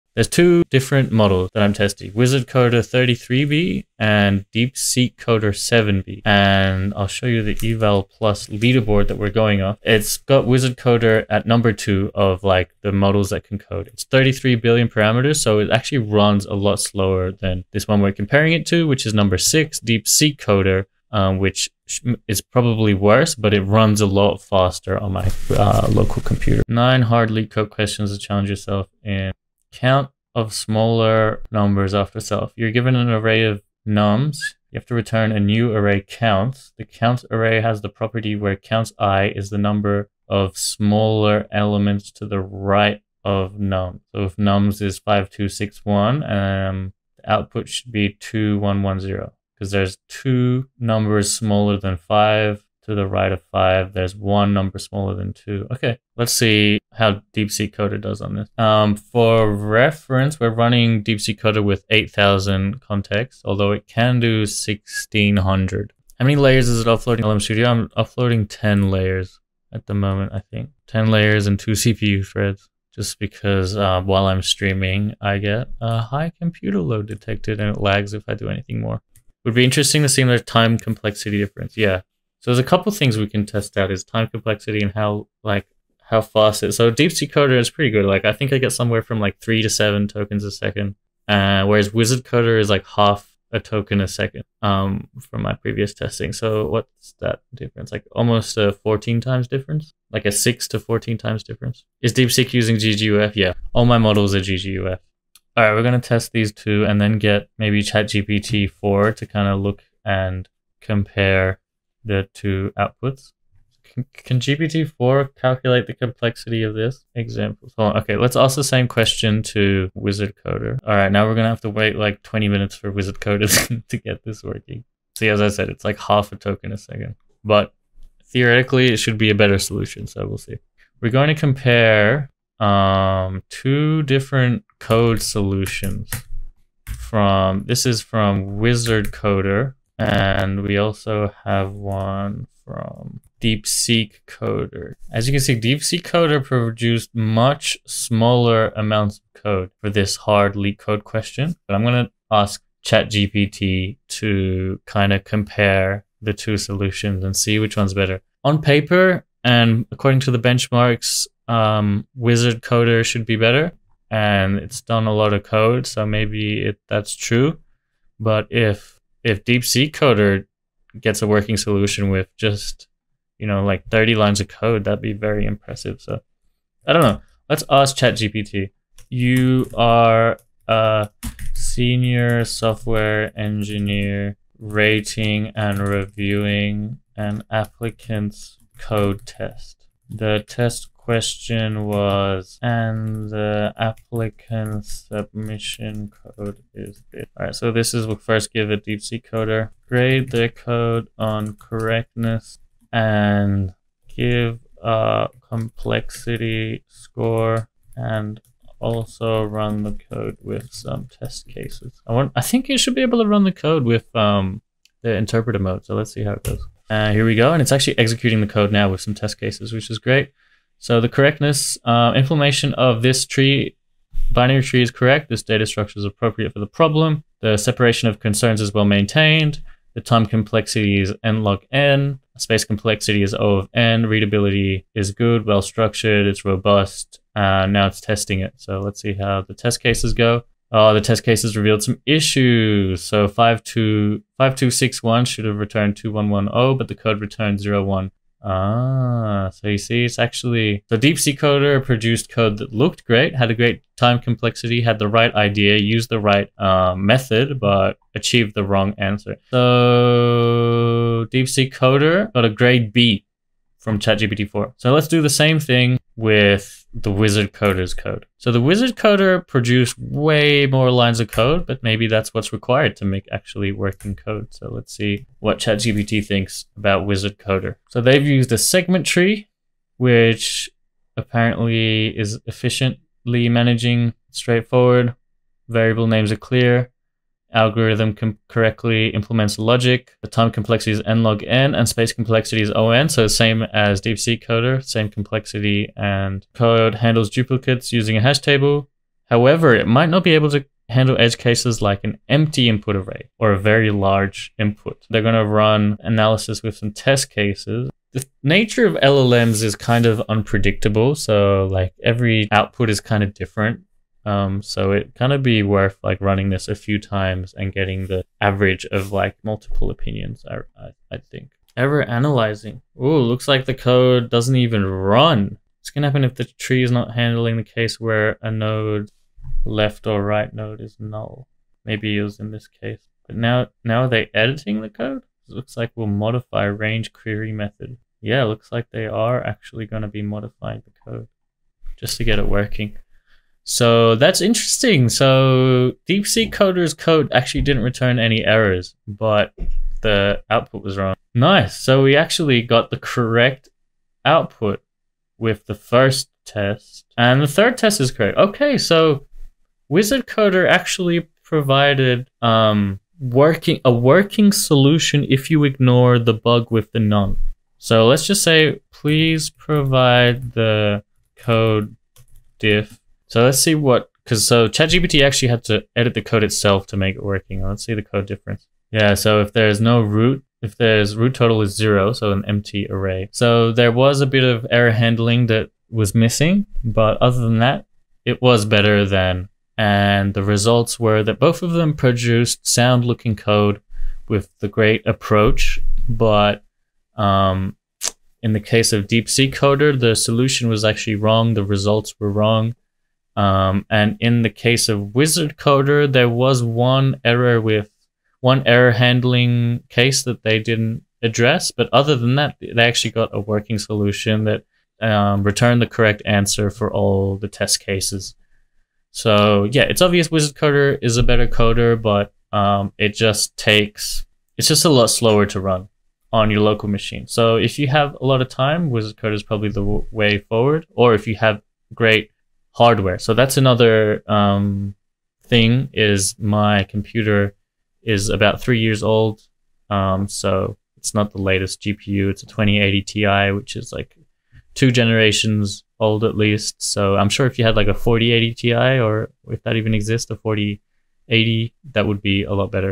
Deepseek 7B vs. Wizard 33B: Coding AIs Showdown on HARD LEETCODE (GPT4 judge)
I compared two AI coding models, DeepSeek Coder 7B and Wizard Coder 33B, using a complex LeetCode question. I noted that despite Wizard Coder's larger size and slower speed, its coding capabilities seemed more advanced. I examined factors like time complexity and efficiency. Using GPT-4 for evaluation, I found that DeepSeek Coder was faster but less accurate, while Wizard Coder, albeit slower, provided a correct solution with some minor errors. My conclusion was that Wizard Coder, though more resource-heavy, outperformed in coding proficiency.